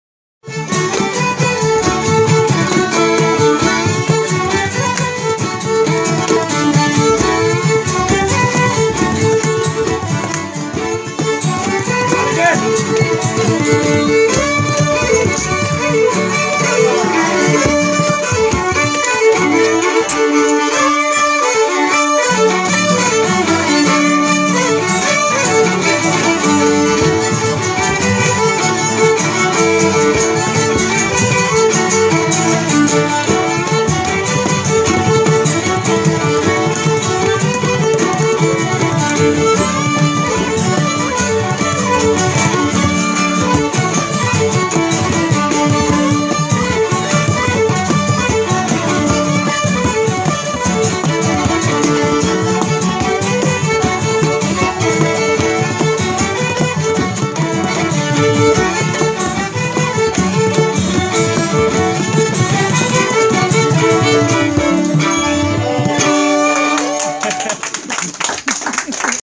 Session irlandaise en Bretagne
Session irlandaise au café-culture Le Cheval Blanc, à Plancoët, le 4 mars 2016